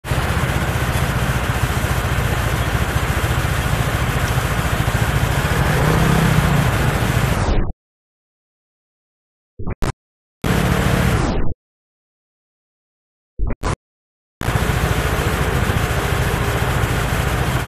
三重日常。
很意外的三重警察效率這麼好，
23號上午送檢舉，24號就收到回信了。
值得鼓勵。